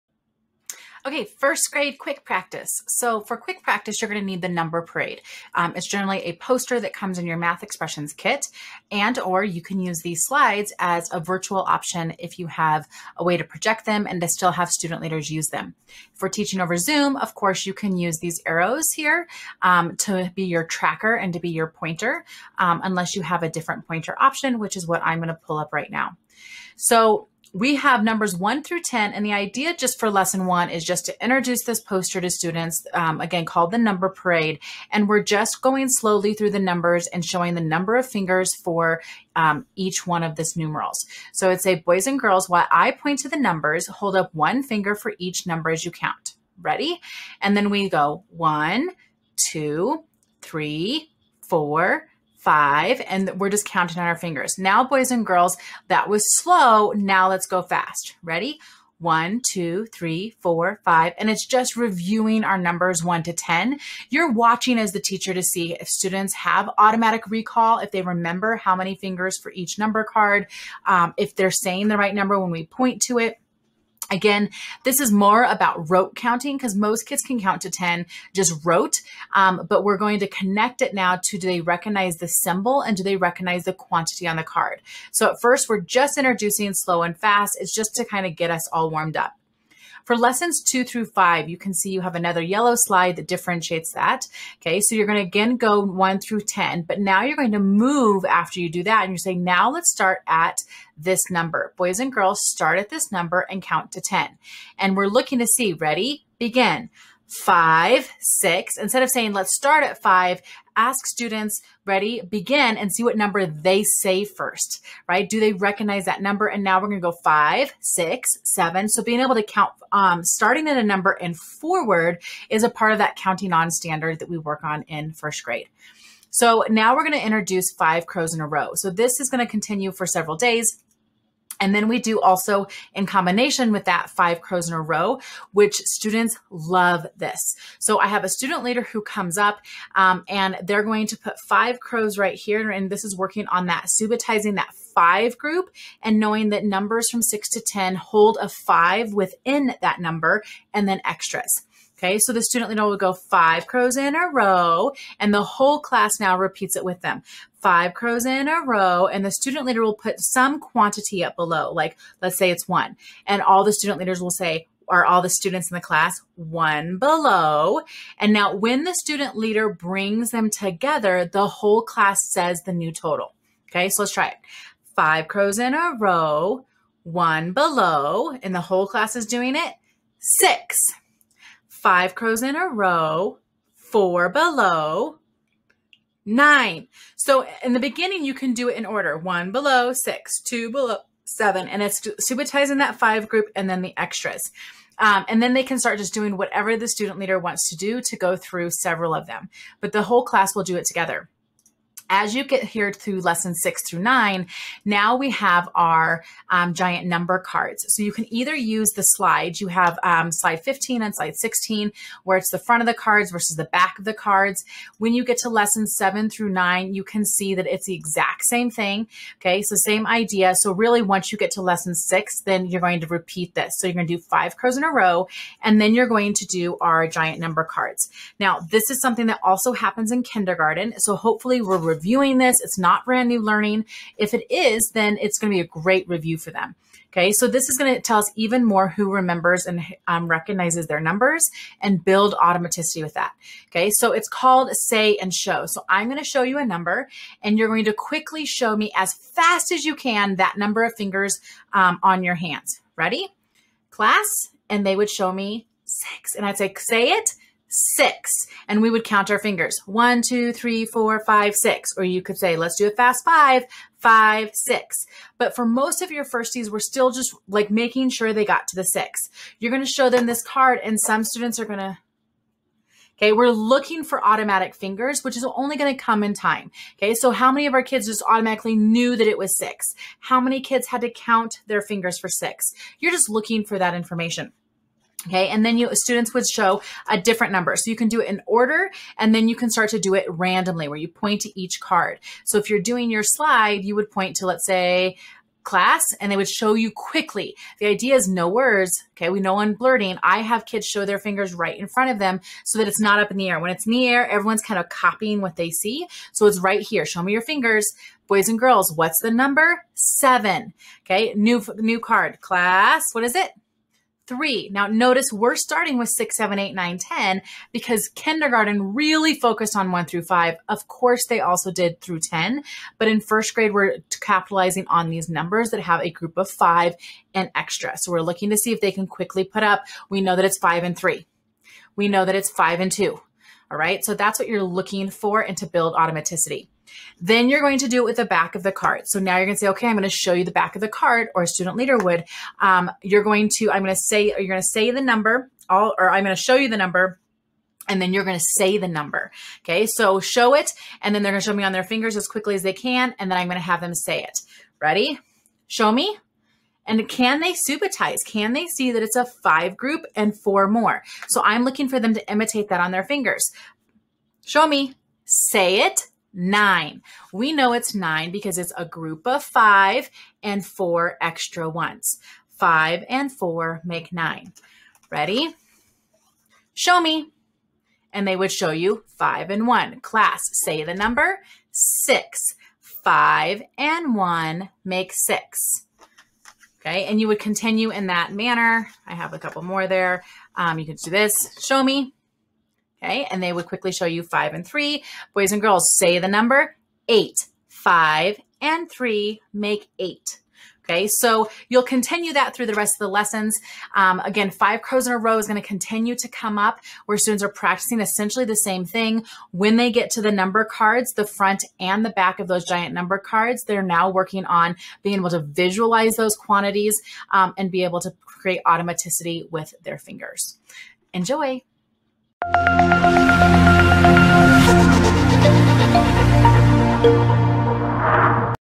First Grade Unit 1 Math Expressions Quick Practices
If you teach Math Expressions in 1st grade, watch this video to see how to implement the Unit 1 Quick Practices.

Teach Math Expressions?